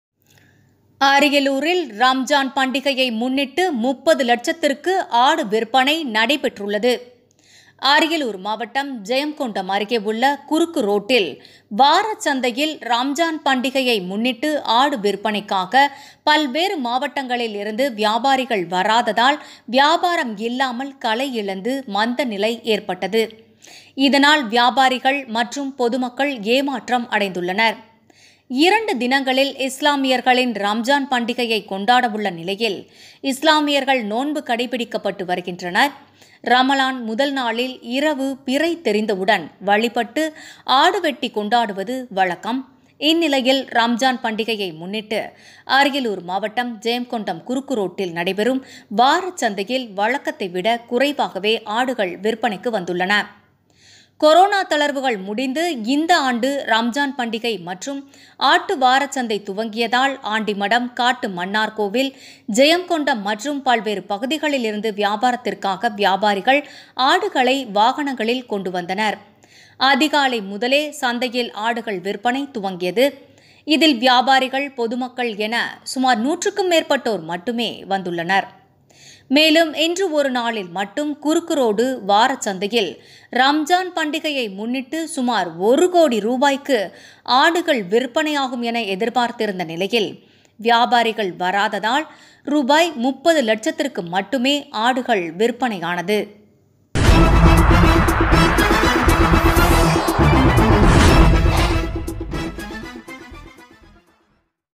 அரியலூரில் ரம்ஜான் பண்டிகையை முன்னிட்டு 30 லட்சத்திற்க்கு ஆடு மந்தமான நிலையில் விற்பனை நடைபெற்றது
அரியலூர் மாவட்டம் ஜெயங்கொண்டம் அருகே உள்ள குறுக்குரோட்டில் வார சந்தையில்  ரம்ஜான் பண்டிகையை முன்னிட்டு ஆடு விற்பனைக்காக   பல்வேறு மாவட்டங்களில் இருந்து  வியாபாரிகள் வராததால் வியாபாரம் இல்லாமல் கலையிழந்து மந்த நிலை ஏற்பட்டது இதனால் வியாபாரிகள் மற்றும் பொதுமக்கள் ஏமாற்றம்.